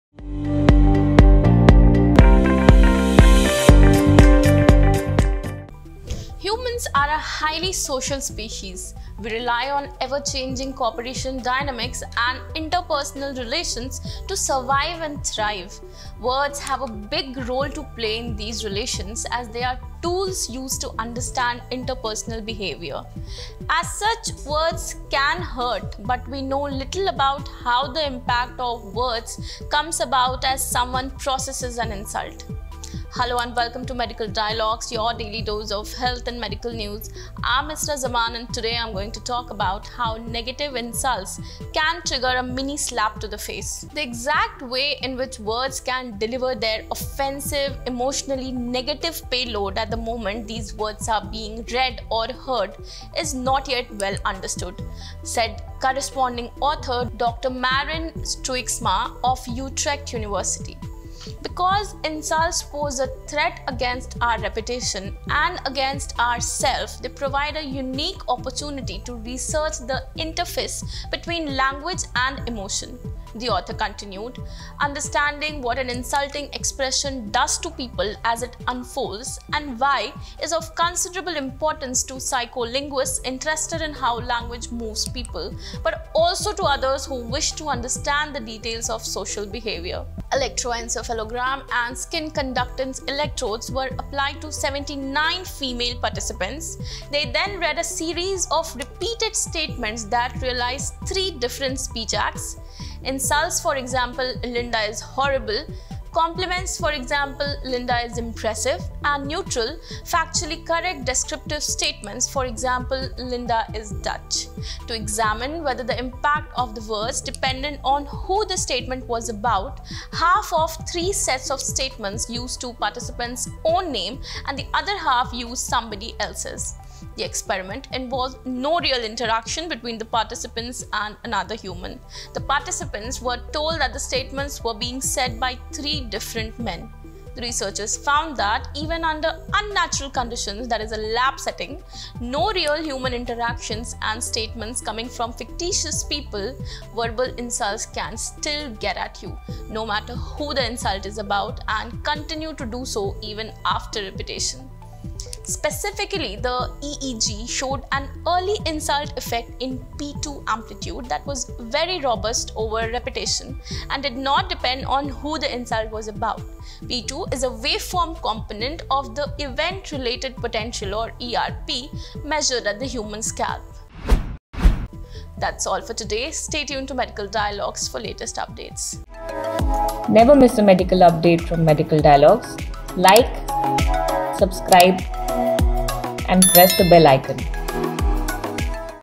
How negative insults can trigger a mini slap to the face
The researchers found that even under unnatural conditions — a lab-setting, no real human interactions, and statements coming from fictitious people — verbal insults can still “get at you”, no matter who the insult is about, and continue to do so even after repetition.
Specifically, the EEG showed an early insult effect in P2 amplitude that was very robust over repetition and did not depend on who the insult was about. P2 is a waveform component of the event-related potential (ERP) measured at the human scalp.